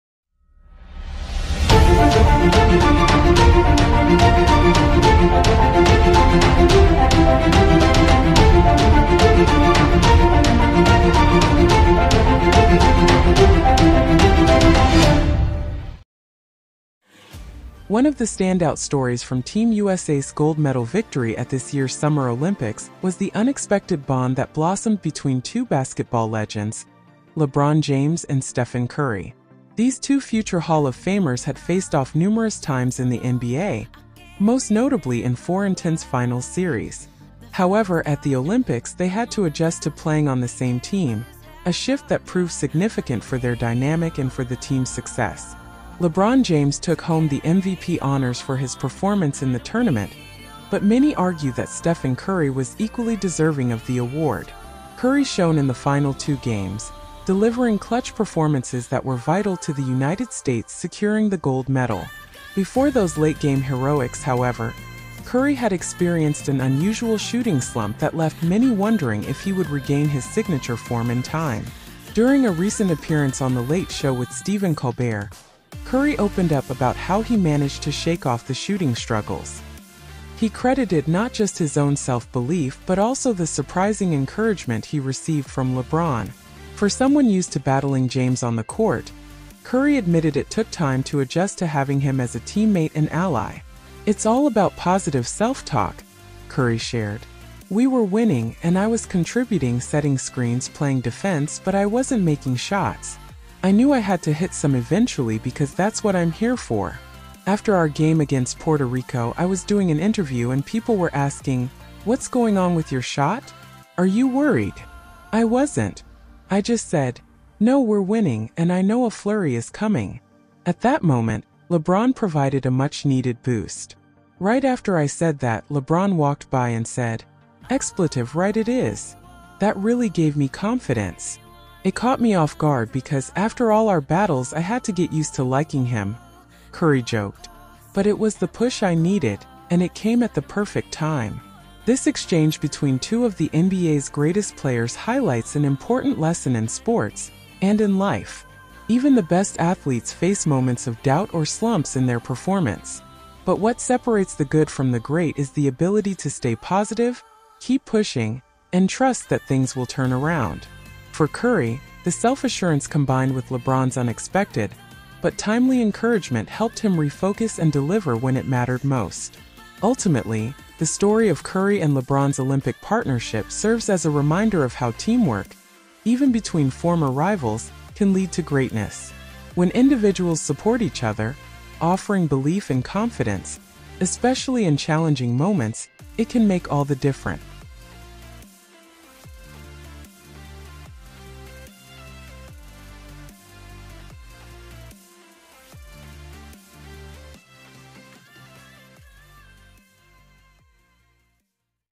HE’S THE BEST! Curry in TEARS As He REVEALS How LeBron Helped Him End Olympic Shooting Slump.
LeBron James, the legendary star of the Los Angeles Lakers, is about to embark on his 22nd season in the NBA. Since stepping onto the court with the Cleveland Cavaliers in 2003, James has built a career full of historic achievements. From becoming the NBA’s all-time leading scorer to winning championships, LeBron has done it all.